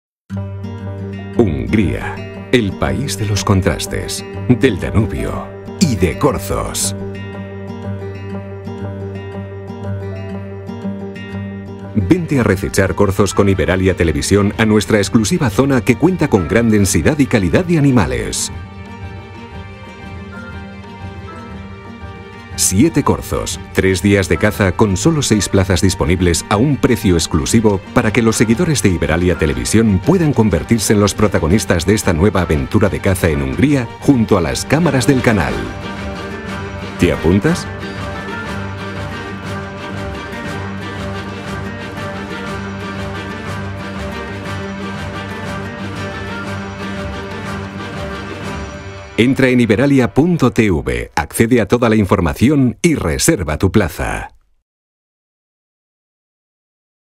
Te apuntas hungria 2020 Nuevas fechas julio
En una exclusiva zona de caza, famosa por su densidad y calidad, Iberalia Expediciones se desplazará a Hungría de la mano de Sierra España, para recechar corzos durante 3 días.

Una ocasión única en la que ofertamos 6 plazas a un precio exclusivo con la única intención de que disfrutes de una auténtica aventura extrema en un territorio incomparable junto a las cámaras del canal.